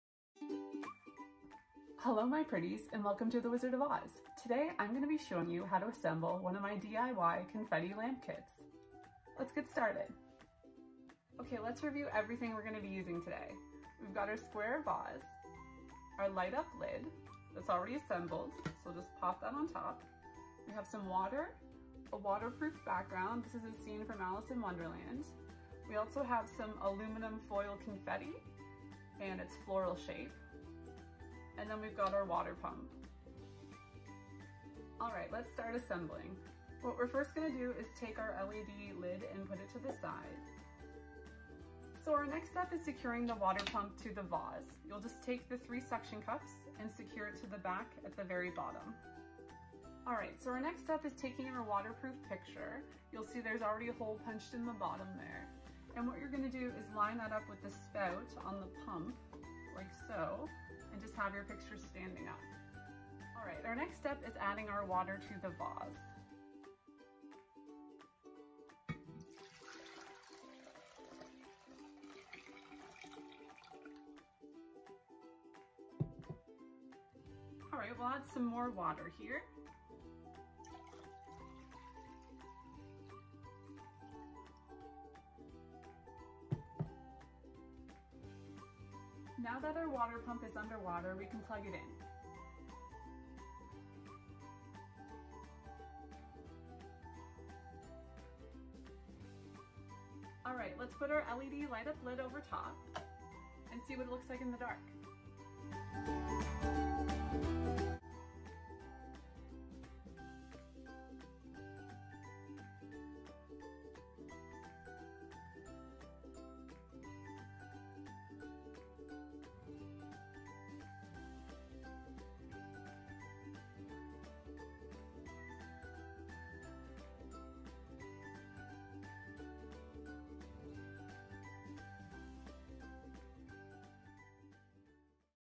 DIY Alice in Wonderland Nightlight (Lamp Kit - THe Wizard of Vase
Watch The Wizard work their magic as they assemble one of their DIY Confetti Lamp Kits.